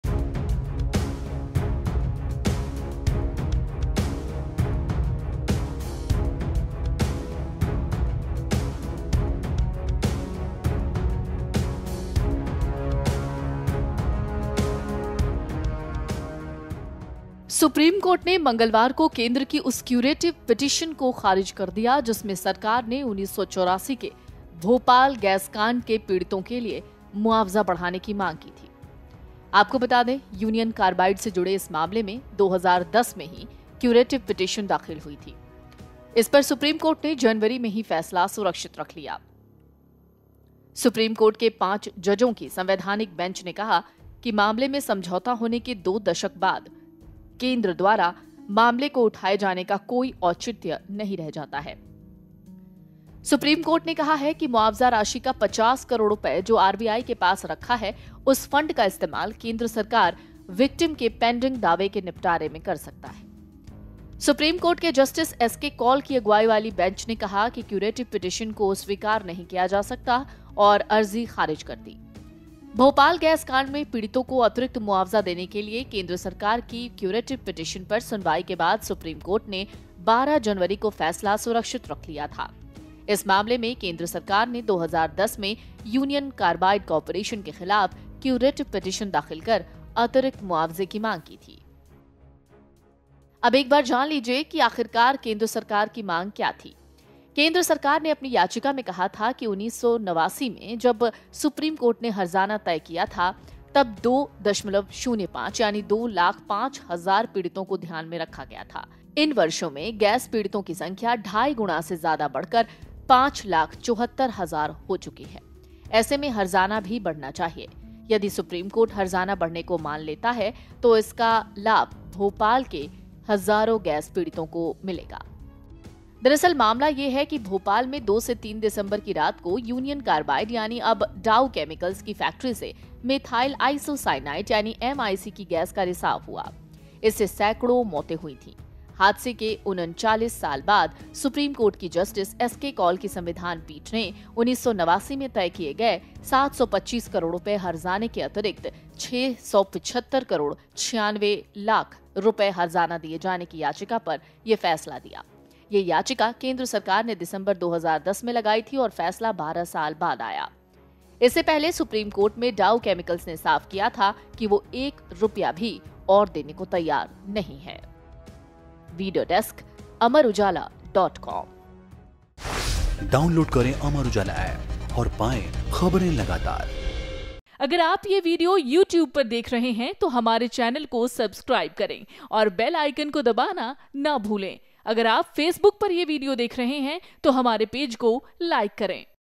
Bhopal Gas Tragedy Case Verdict: पीड़ितों का नहीं बढ़ेगा मुआवजा, जानें Supreme Court में क्या हुआ
सुप्रीम कोर्ट ने मंगलवार को भोपाल गैस पीड़ितों के लिए यूनियन कार्बाइड कॉर्पोरेशन और उसकी सहायक फर्माें से अतिरिक्त मुआवजा दिलाने की केंद्र सरकार की याचिका को खारिज कर दिया। केंद्र ने 2010 में क्यूरेटिव पिटीशन के जरिए डाउ कैमिकल्स से 7800 करोड़ का अतिरिक्त मुआवजा दिलाने की अपील की थी।